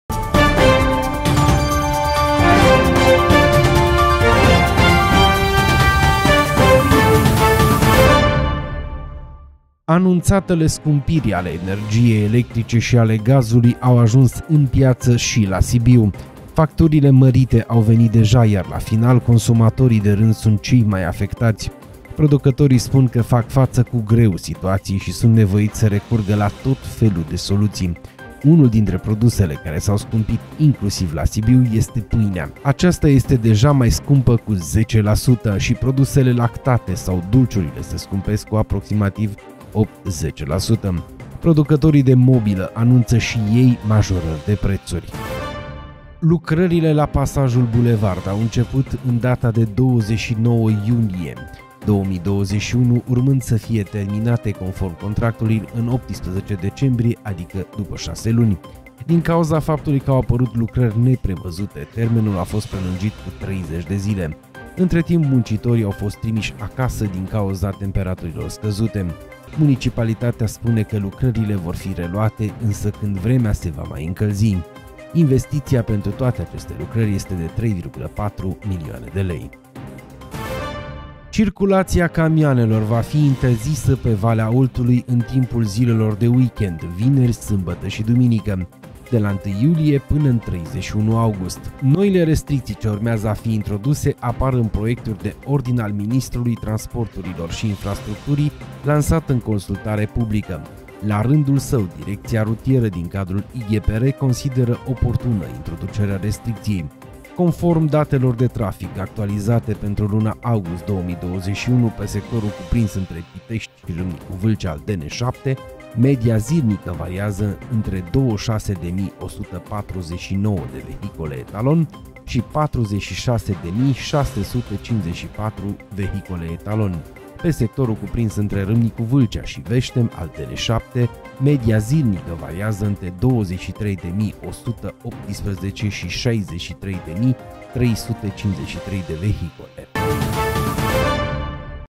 Știrile zilei la Sibiu 🗞Scumpiri la Sibiu – Și pâinea costă mai mult 🥖
Scumpiri la Sibiu – Și pâinea costă mai mult 🥖

Lucrări oprite la Pasajul Bulevard👷🏻

Camioanele interzise în weekend-urile de vară pe Valea Oltului🚚